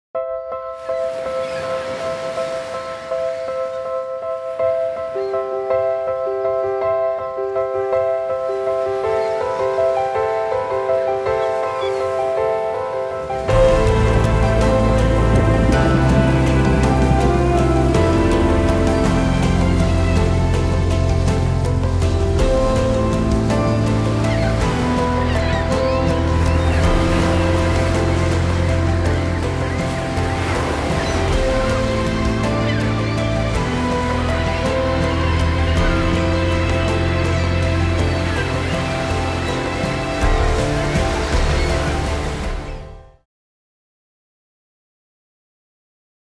Dunas costeras: valiosas fuentes de agua
En el Día Mundial del Agua, valoremos aquellos ecosistemas que son fuentes del líquido vital, como las DunasCosteras.